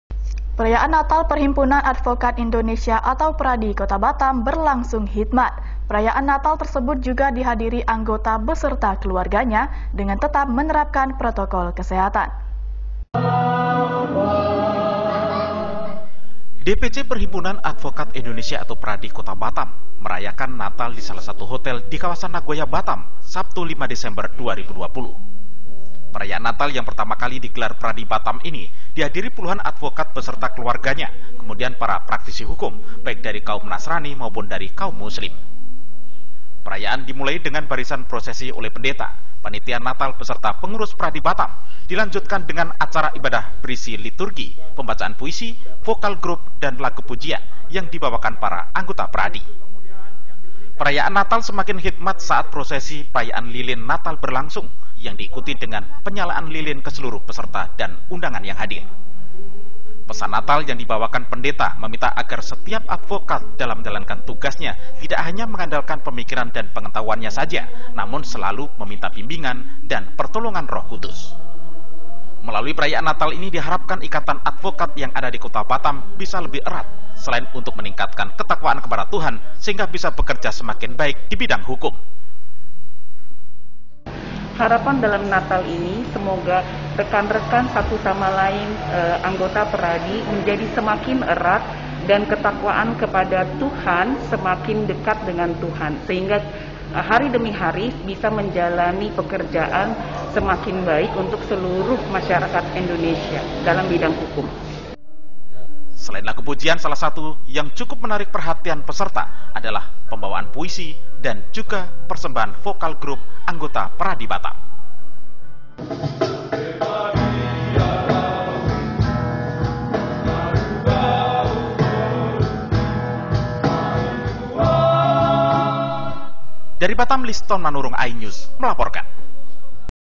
PERAYAAN NATAL PERADI BATAM BERLANGSUNG KHITMAD- Seputar Inews Kepri
PERAYAAN NATAL  PERHIMPUNAN ADVOKAT INDONESIA ATAU PERADI  KOTA BATAM BERLANGSUNG KHITMAD// PERAYAAN NATAL TERSEBUT JUGA DIHADIRI   ANGGOTA BESERTA KELUARGANYA/ DENGAN TETAP MENERAPKAN PTOROKOL KESEHATAN//